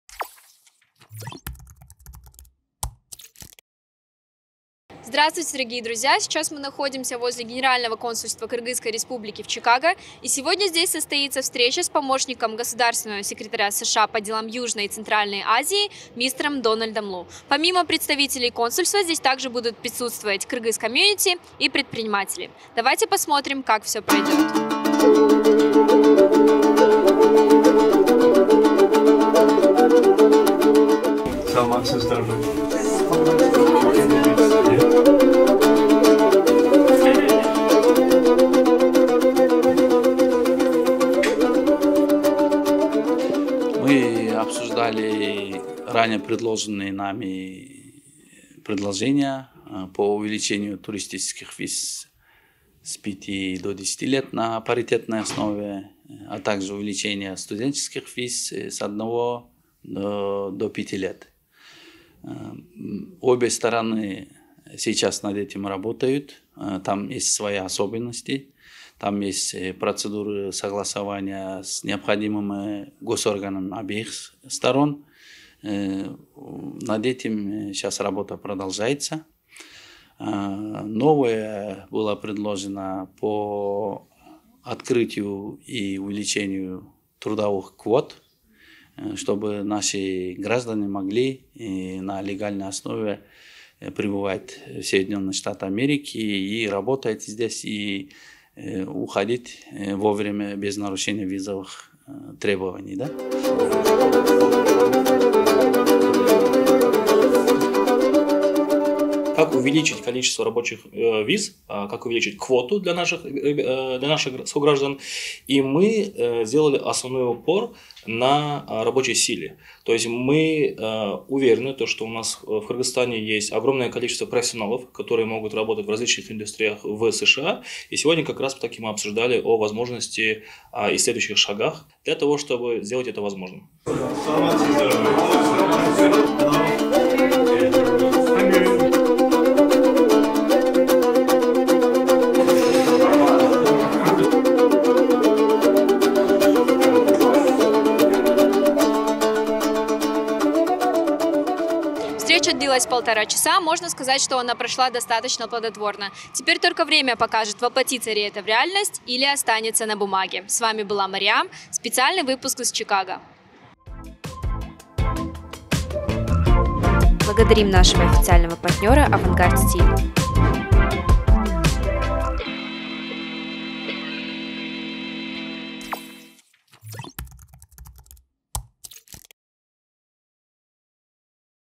В Чикаго обсудили вопросы рабочих виз в США для кыргызстанцев
На днях в Чикаго состоялась встреча госсекретаря США по делам Южной и Центральной Азии Дональда Лу и генерального консула Болота Борбиева. На встрече обсуждались вопросы продления туристической, журналистской, учебной виз, а также возможность открытия рабочих виз для граждан Кыргызстана. Тимур Тиленов, кыргызский бизнесмен в США, выступил с 20-минутной презентацией и внес предложения по привлечению рабочих из Кыргызстана.